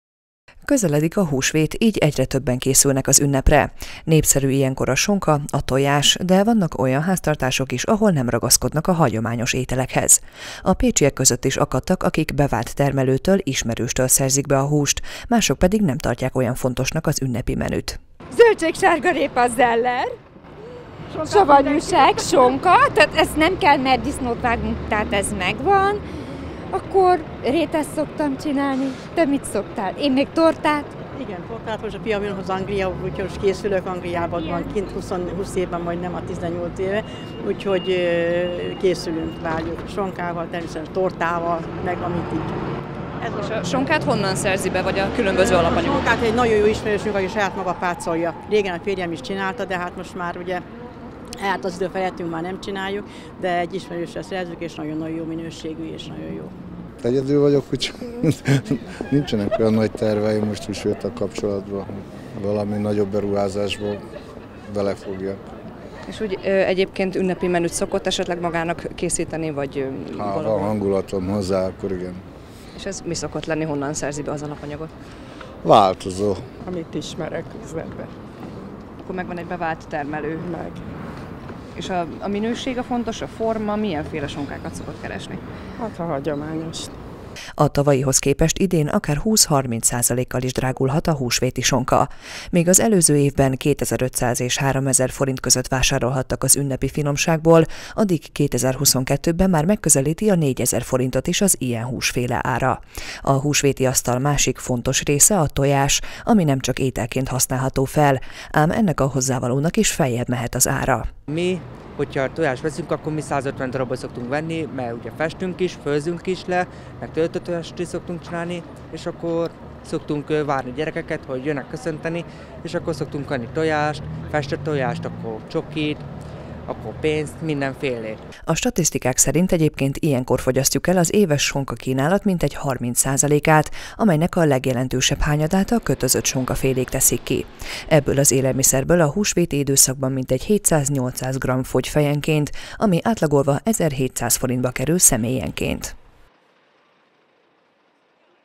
Húsvéti szokások
A sonka igen előkelő helyet foglal el a húsvéti ünnepi asztalon. Népszerű még ilyenkor a tojás is, ám mindkét élelmiszer tekintetében drágulással kell számolni – mondják a szakemberek. Megkérdeztük a pécsieket, ők készülnek-e ünnepi menüvel és milyen szempont alapján szerzik be az élelmiszert.